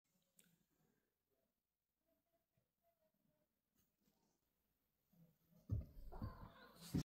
Mrrp
Oreo Mrrps, when he sees a visitor outside, he loves meeting new people.